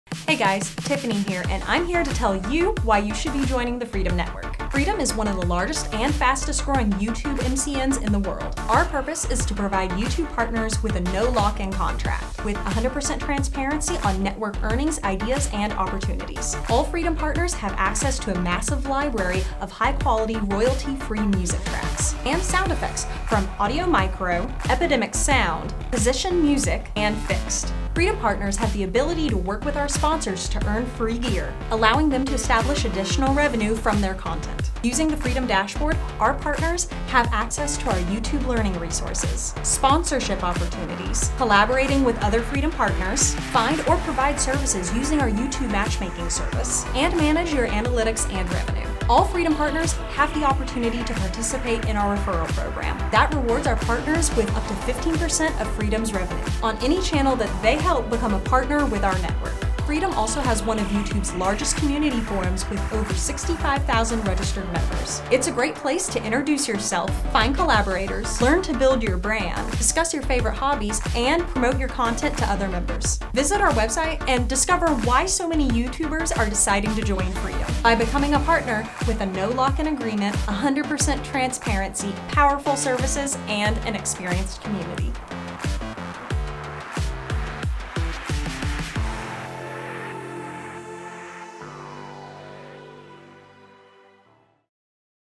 ★ Welcome to Freedom! - No lock-in contracts - Exclusive music - Community
How does Freedom! help you grow? - Tifani explains the ways you can grow faster with Freedom!

Our purpose is to provide YouTube partners with a no lock-in contract with 100% transparency on network earnings, ideas and opportunities.

Partners have access to our YouTube learning resources, sponsorship opportunities, collaborate with other Freedom partners, find or provide video services using our matchmaking feature and manage your analytics and revenue.

Our referral program that rewards our partners with 15% of Freedom's revenue on any channel they help become a partner with our network.

Freedom! also has one of the largest YouTube community forums in the world with over 65,000 registered members. It's a great place to introduce yourself, find collaborators, learn ways to build your brand, discuss your favorite hobbies and promote your content to other members.

▼ Links

▼ Heartbeat

▼ Freedom!

▼ Music